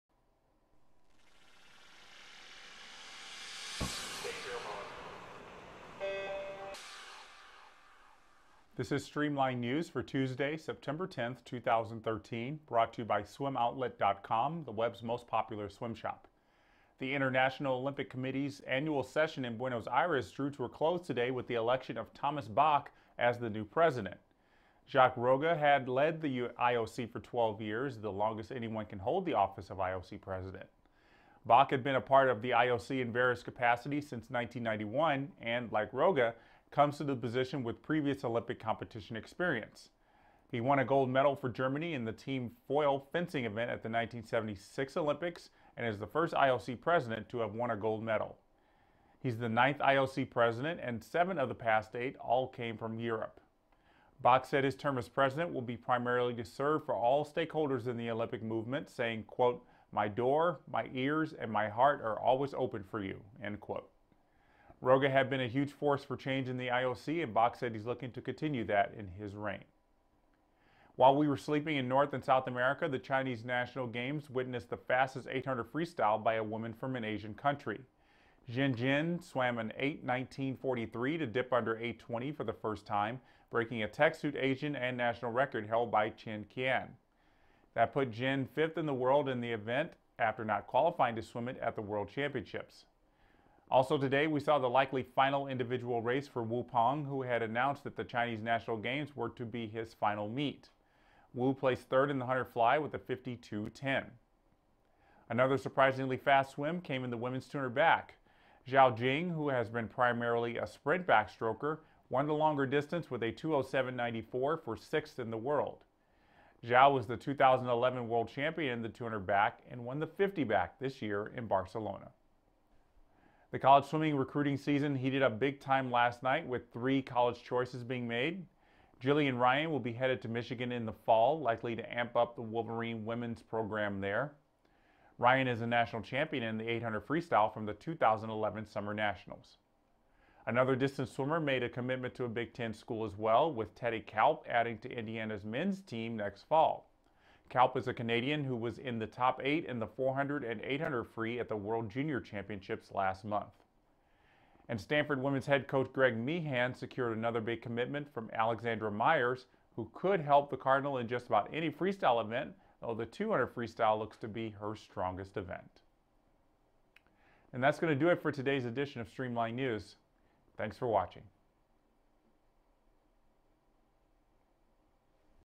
Streamlined News: Headlines from IOC, China, US College
New IOC president picked; Asian record broken in women's 800 free; College recruits pick schools.